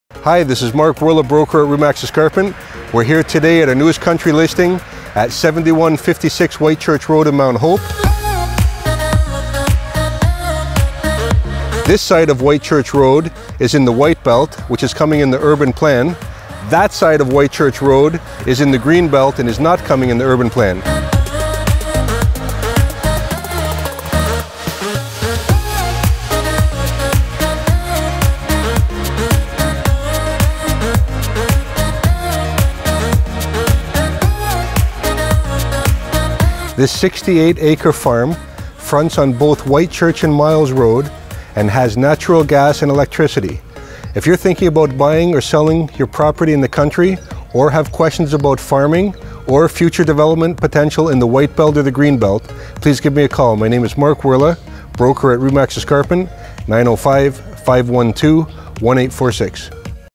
7156 White Church Road East - 68 +/- ACRES - FOR SALE - FUTURE DEVELOPMENT WHITE BELT ZONE.
WHITE BELT Future Development property, as identified in the City of Hamilton Official Plan. 68 +/- acres, fronting on White Church Rd and Miles Rd. This property is in the WHITE Belt (NOT Green Belt). This property features a beautiful home, large barn, and is presently used as a tree farm & cash crop. Please see the photos for more information. Buyers to complete do due diligence before sending offer.

Mark Woehrle
Broker
RE/MAX Escarpment Realty Inc.

Wendy Tanner
Broker

Nancy Neville
Realtor

Darla Youldon
Realtor

Dagmar Chindemi
Realtor